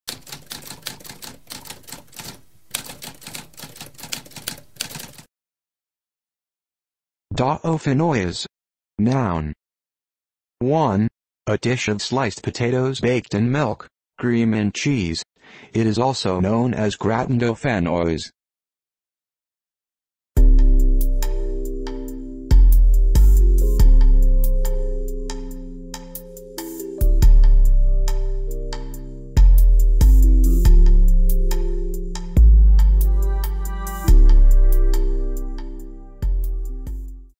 What does dauphinoise mean?
A spoken definition of dauphinoise.

Intro Sound:
Typewriter - Tamskp